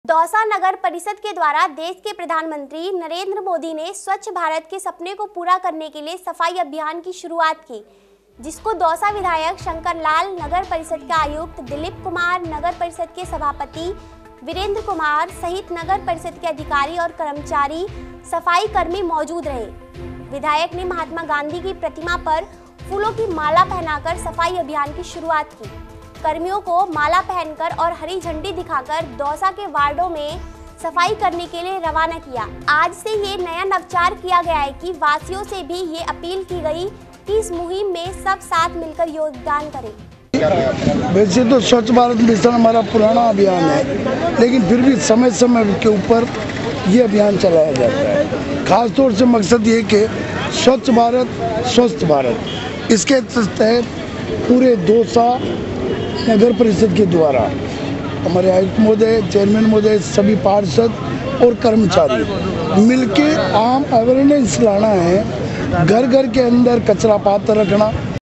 दौसा ║ नगर परिषद् नें सफाई अभियान की शुरुआत की
bvmnews haryananews topnews bvmnewslive latestnews दौसा नगर परिषद् के द्वारा देश के प्रधानमंत्री नरेंद्र मोदी ने स्वच्छ भारत के सपने को पूरा करने के लिए सफाई अभियान की शुरुआत की हे जिसको दौसा विधायक शंकर लाल नगर परिषद् के आयुक्त दिलीप कुमार नगर परिषद् के सभापति वीरेंदर कुमार सहित नगर परिषद् के अधिकारी और कर्मचारी एव सफाई कर्मी मौजूद रहे विधायक ने महात्मा गाँधी की प्रतिमा पर फूलो की माला पहना कर सफाई अभियान की शुरुआत की कर्मियों को माला पहनकर और हरी झंडी दिखाकर दौसा के वार्डो में सफाई करने के लिए रवाना किया आज से ये नया नवाचार किया हे ज़िले वासियो से भी ये अपील की गई कि इस मुहीम में सब साथ मिल कर योगदान करे